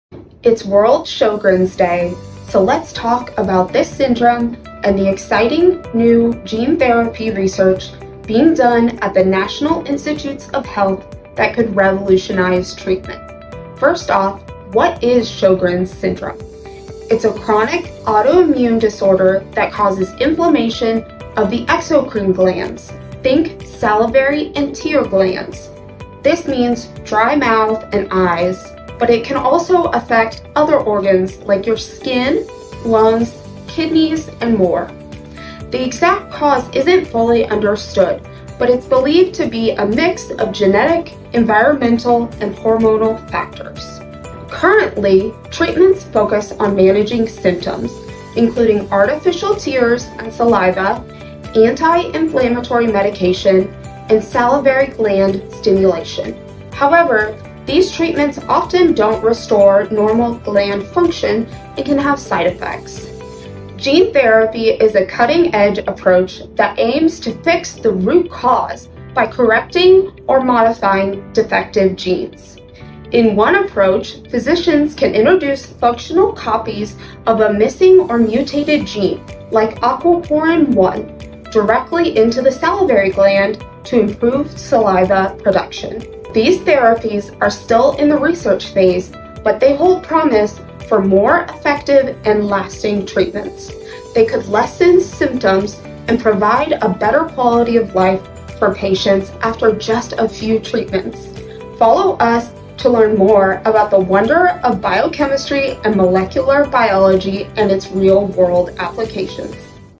What is Sjögren's syndrome?
In observance of 2024 World Sjögren's Day, ASBMB science writer Marissa Locke Rottinghaus describes the syndrome and cutting-edge gene therapy treatments to improve tear and saliva production.